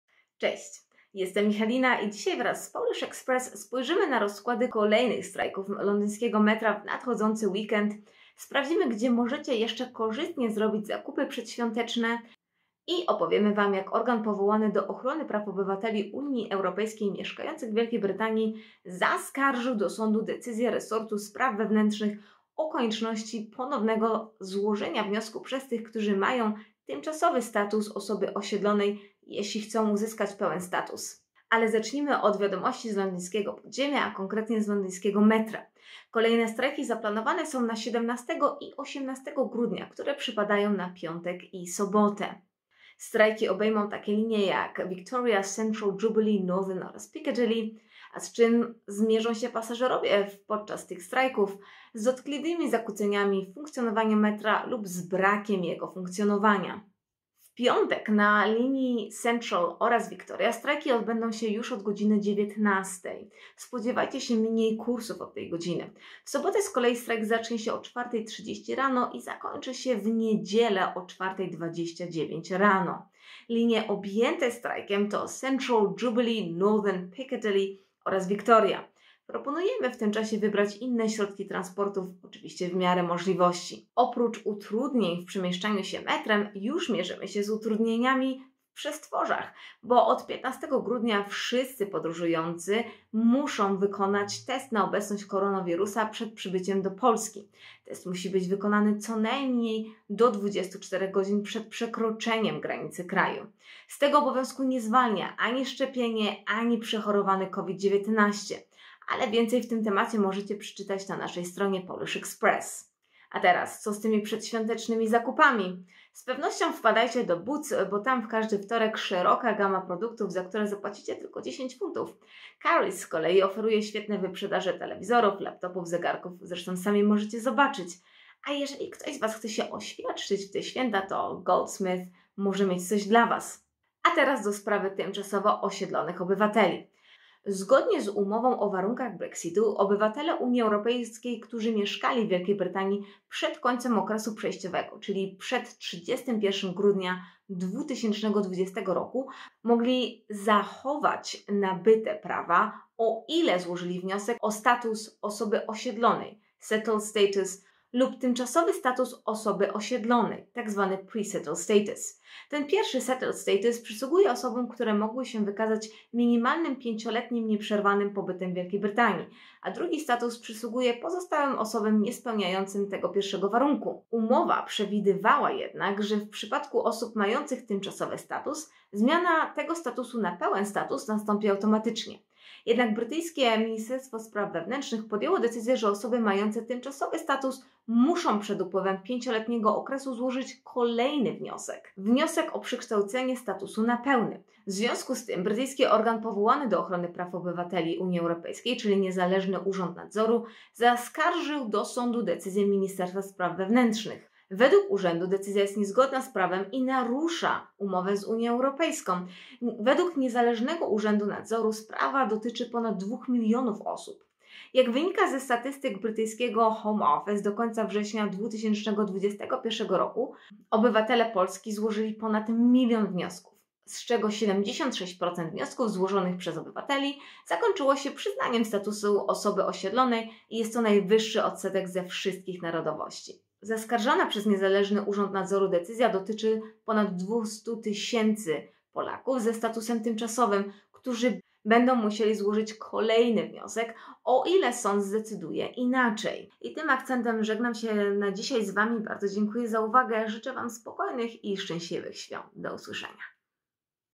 POLISH EXPRESS: strajki metra w Londynie, Boże Narodzenie 2021 oraz status osiedlonego
O czym powinni wiedzieć mieszkający w UK Polacy? Oto podsumowanie z komentarzem - zapraszamy! Więcej codziennych informacji